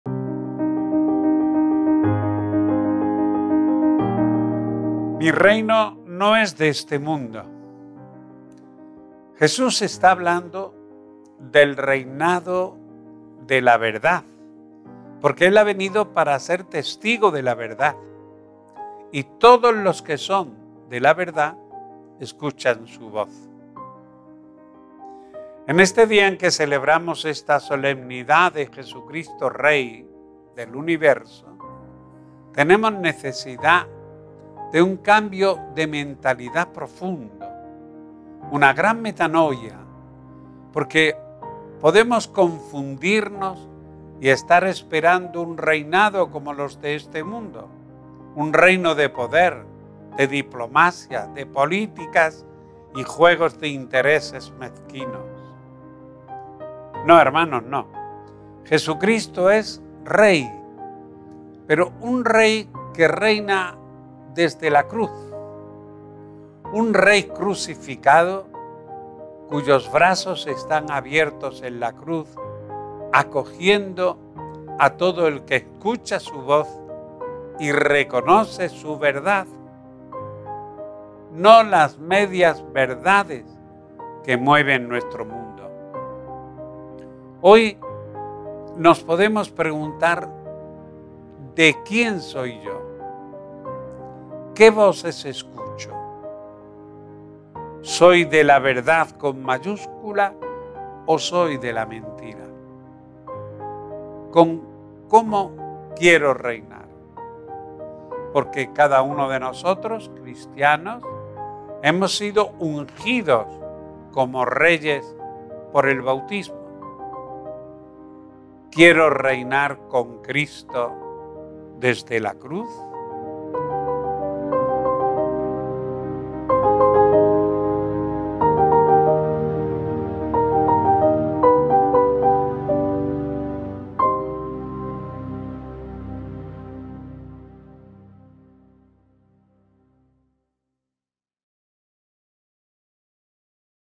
24 - 11 - 2024 | ¿Quiero reinar con Cristo desde la cruz?
“Mi reino no es de este mundo”. Jesús está hablando del reinado de la verdad, porque Él ha venido para ser testigo de la verdad, y todos los que son de la verdad escuchan su voz. En este día en que celebramos esta Solemnidad de Jesucristo Rey del Universo, tenemos necesidad de un cambio de mentalidad profundo, una gran metanoia, porque podemos confundirnos y estar esperando un reinado como los de este mundo, un reino de poder, de diplomacia, de políticas y juegos de intereses mezquinos. No hermanos, no, Jesucristo es Rey, pero un rey que reina desde la cruz, un Rey Crucificado, cuyos brazos están abiertos en la cruz, acogiendo a todo el que escucha su voz y reconoce Su verdad, no las medias verdades que mueven nuestro mundo. Hoy nos podemos preguntar ¿de quién soy yo? ¿Qué voces escucho? ¿Soy de la Verdad con mayúscula o soy de la mentira? ¿Cómo quiero reinar? Porque cada uno de nosotros, cristianos, hemos sido ungidos como reyes por el bautismo. ¿Quiero reinar con Cristo desde la cruz?